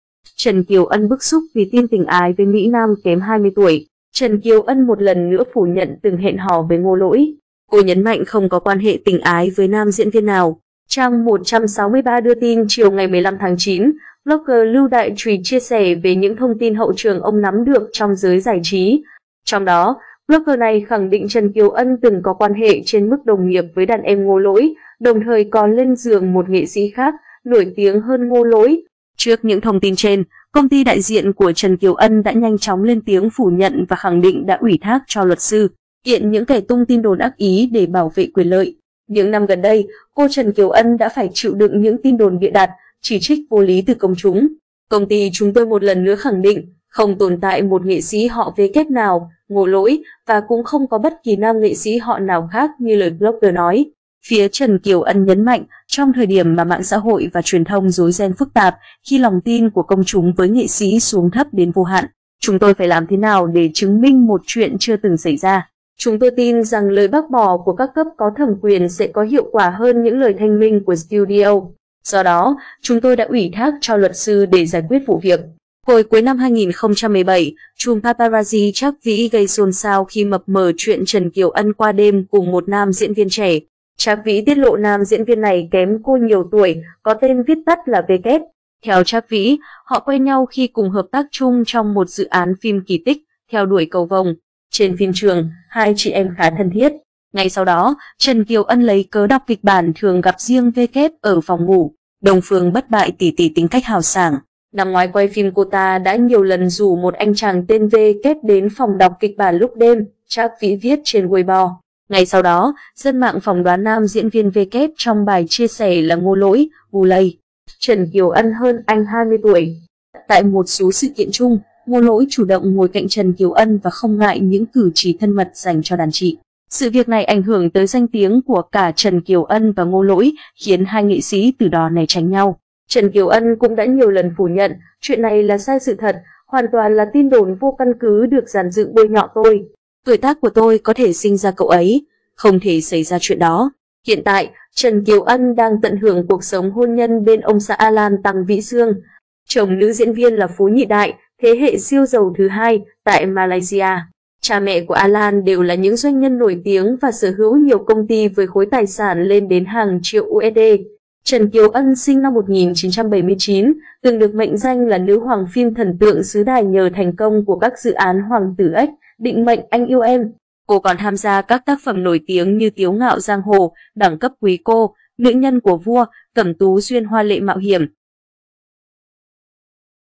Trần Kiều Ân bức xúc vì tin tình ái với mỹ nam kém 20 tuổi
Trần Kiều Ân một lần nữa phủ nhận từng hẹn hò với Ngô Lỗi. Cô nhấn mạnh không có quan hệ tình ái với nam diễn viên nào.Theo Trác Vỹ, họ quen nhau khi cùng hợp tác chung trong một dự án phim Kỳ tích: Theo đuổi cầu vồng. Trên phim trường, hai chị em khá thân thiết. Ngay sau đó, Trần Kiều Ân lấy cớ đọc kịch bản thường gặp riêng W ở phòng ngủ.
 Hai nghệ sĩ bị đồn hẹn hò vì có nhiều cử chỉ thân thiết. Ảnh: Sina.
Trang 163 đưa tin chiều 15/9, blogger Lưu Đại Chùy chia sẻ về những thông tin hậu trường ông nắm được trong giới giải trí. Trong đó, blogger này khẳng định Trần Kiều Ân từng có quan hệ trên mức đồng nghiệp với đàn em Ngô Lỗi, đồng thời còn lên giường một nghệ sĩ khác, nổi tiếng hơn Ngô Lỗi.
"Những năm gần đây, cô Trần Kiều Ân đã phải chịu đựng những tin đồn bịa đặt, chỉ trích vô lý từ công chúng. Công ty chúng tôi một lần nữa khẳng định, không tồn tại một nghệ sĩ họ W nào (Ngô Lỗi), và cũng không có bất kỳ nam nghệ sĩ họ nào khác như lời blogger nói".
"Đông Phương Bất Bại tỷ tỷ tính cách hào sảng. Năm ngoái quay phim cô ta đã nhiều lần rủ một anh chàng tên W đến phòng đọc kịch bản lúc đêm", Trác Vỹ viết trên Weibo.
Hồi cuối năm 2017, “trùm paparazzi” Trác Vỹ gây xôn xao khi mập mờ chuyện Trần Kiều Ân qua đêm cùng một nam diễn viên trẻ. Trác Vỹ tiết lộ nam diễn viên này kém cô nhiều tuổi, có tên viết tắt là W.
Trần Kiều Ân sinh năm 1979, từng được mệnh danh là nữ hoàng phim thần tượng xứ Đài nhờ thành công của các dự án Hoàng tử ếch, Định mệnh anh yêu em. Cô còn tham gia các tác phẩm nổi tiếng như Tiếu ngạo giang hồ, Đẳng cấp quý cô, Nữ nhân của vua, Cẩm Tú Duyên hoa lệ mạo hiểm...
Hiện tại, Trần Kiều Ân đang tận hưởng cuộc sống hôn nhân bên ông xã Alan Tăng Vỹ Xương. Chồng nữ diễn viên là phú nhị đại (thế hệ siêu giàu thứ hai) tại Malaysia. Cha mẹ của Alan đều là những doanh nhân nổi tiếng và sở hữu nhiều công ty với khối tài sản lên đến hàng triệu USD.
Scandal tình ái của Trần Kiều Ân và Ngô Lỗi lại bị nhắc lại. Ảnh: Weibo.
Trước những thông tin trên, công ty đại diện của Trần Kiều Ân đã nhanh chóng lên tiếng phủ nhận và khẳng định đã ủy thác cho luật sư, kiện những kẻ tung tin đồn ác ý để bảo vệ quyền lợi.
Phía Trần Kiều Ân nhấn mạnh: "Trong thời điểm mà mạng xã hội và truyền thông rối ren phức tạp, khi lòng tin của công chúng với nghệ sĩ xuống thấp đến vô hạn, chúng tôi phải làm thế nào để chứng minh một chuyện chưa từng xảy ra? Chúng tôi tin rằng lời bác bỏ của các cấp có thẩm quyền sẽ có hiệu quả hơn những lời thanh minh của studio. Do đó, chúng tôi đã ủy thác cho luật sư để giải quyết vụ việc".
Sự việc này ảnh hưởng tới danh tiếng của cả Trần Kiều Ân và Ngô Lỗi, khiến hai nghệ sĩ từ đó né tránh nhau. Trần Kiều Ân cũng đã nhiều lần phủ nhận: "Chuyện này là sai sự thật, hoàn toàn là tin đồn vô căn cứ được dàn dựng bôi nhọ tôi. Tuổi tác của tôi có thể sinh ra cậu ấy. Không thể xảy ra chuyện đó".
Ngay sau đó, dân mạng phỏng đoán nam diễn viên W trong bài chia sẻ là Ngô Lỗi (Wu Lei). Trần Kiều Ân hơn anh 20 tuổi. Tại một số sự kiện chung, Ngô Lỗi chủ động ngồi cạnh Trần Kiều Ân và không ngại những cử chỉ thân mật dành cho đàn chị.